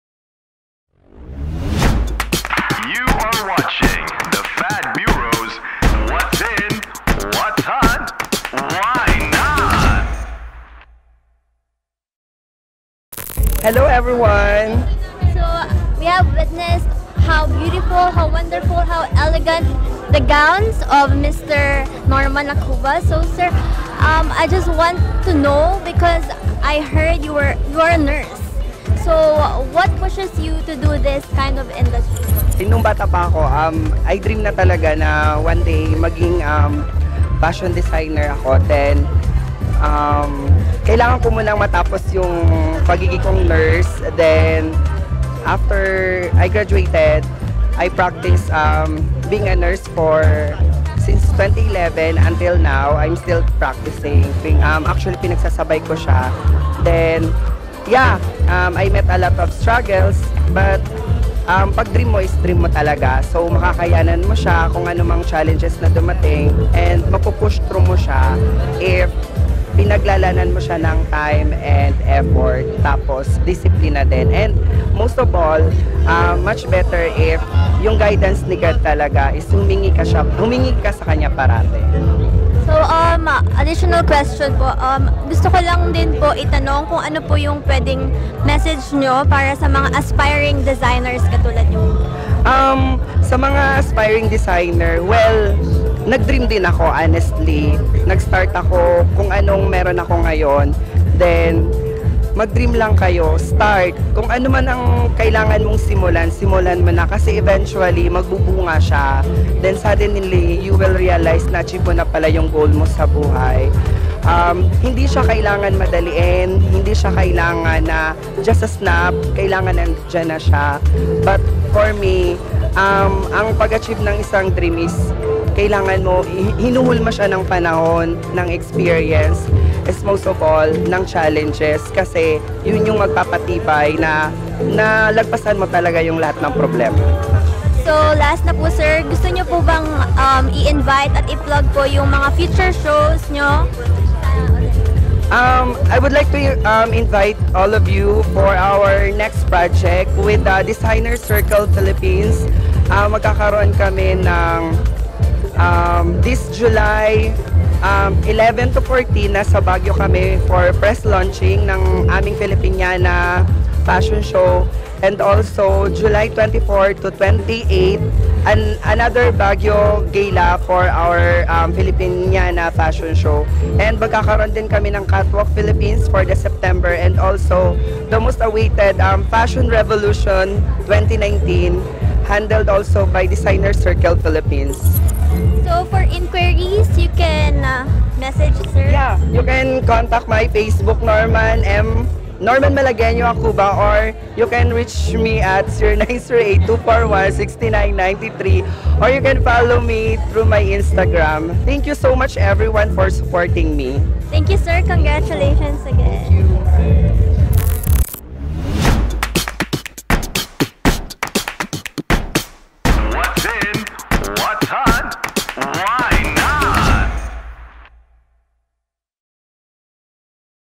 Philippine Fashion Mélange | Interview with NORMAN ACUBA - 06.29.2019
We have witnessed AWESOME designs from some of the best gown designers in the country today. Last June 29, 2019 our new host Ms. Mecelle Miranda got a chance to interview Mr. Norman Acuba who shared his wedding design collections at the Philippine Fashion Mélange presented by the Black Box Studio.